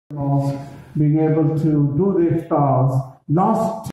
Faith Network for Manchester Ramadhan Iftar at Khizra Mosque on 27/09/1443 (28/04/2022)(1)
إفطار جماعي لكل الديانات في مانشستر في مسجد الخضر في مانشستر بتنظيم شبكة الديانات في مانشستر يوم 27 رمضان 14430الموافق إلى 28 أبريل 2022.